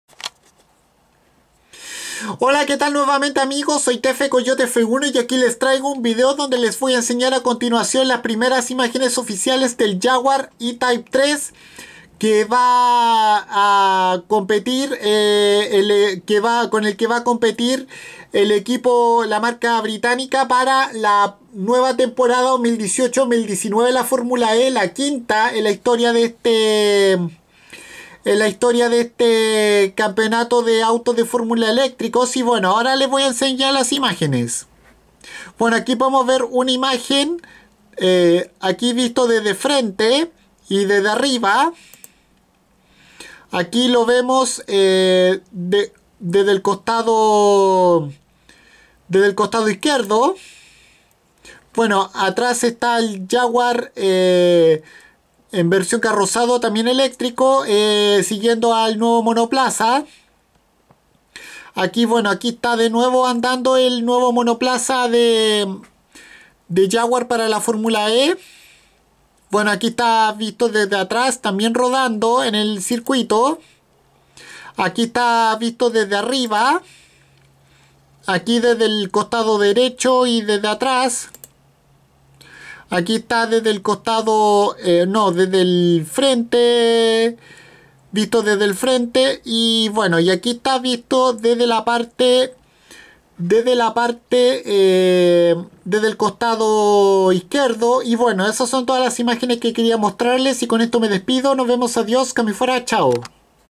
Primeras imágenes oficiales del Jaguar I Type 3 de Formula E
Estas son las primeras fotos oficiales del nuevo monoplaza eléctrico de la marca británica para la nueva Temporada 2018/2019 de la Formula E